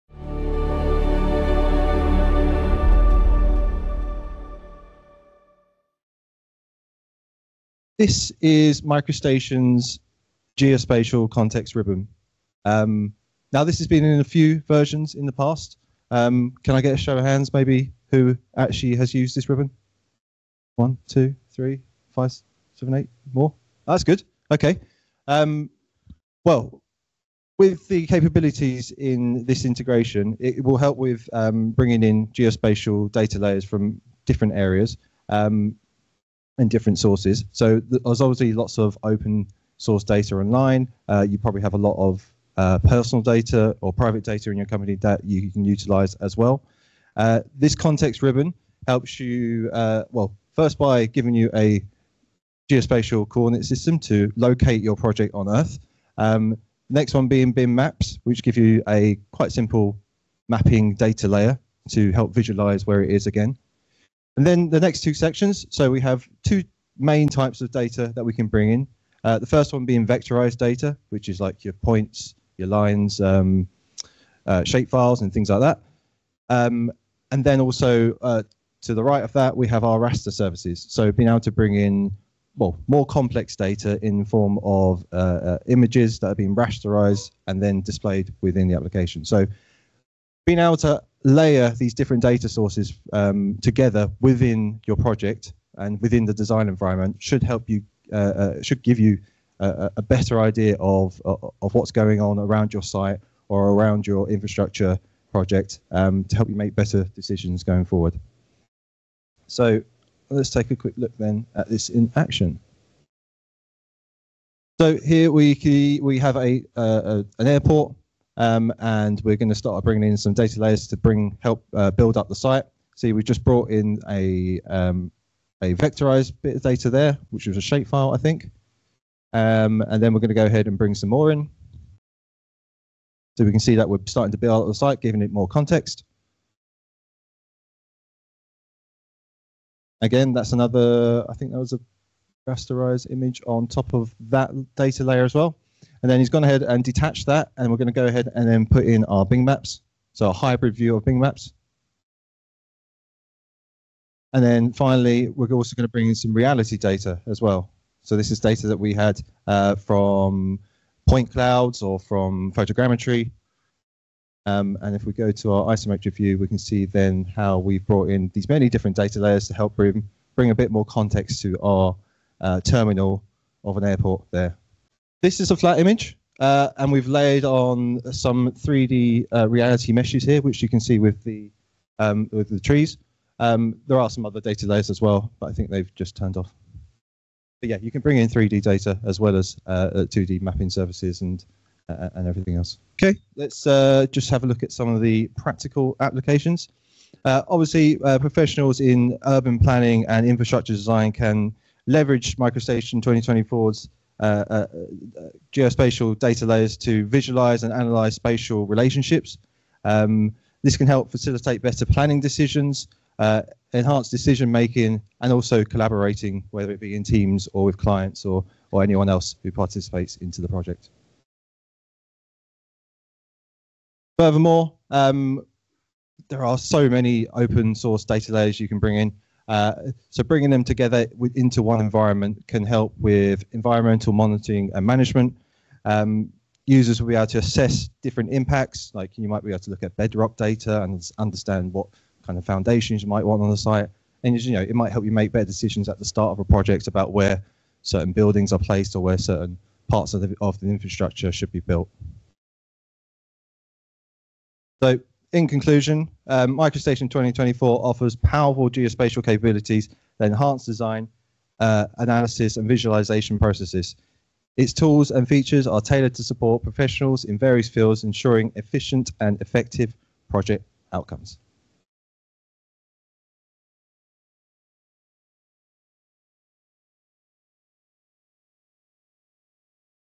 Innovation Meet-Up Series: MicroStation’s Geospatial
Watch Bentley’s Matt Felton explain the benefits of MicroStation’s Geospatial capabilities.